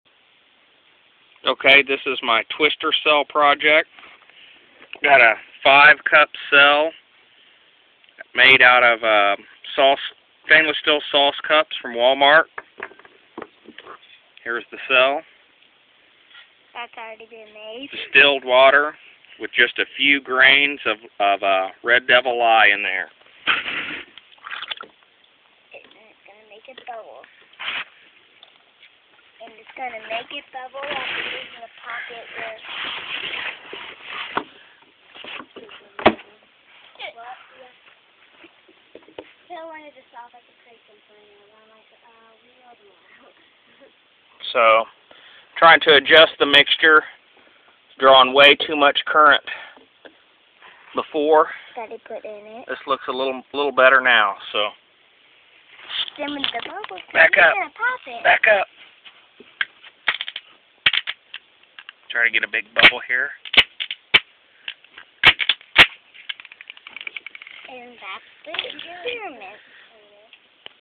HHO Sauce cup electrolyzer
This is my first shot at a HHO cell. Modeled after the "Twister Cell".  5 cup cell.  Still experimenting with spacing and electrolyte.  I have not measured output or current yet.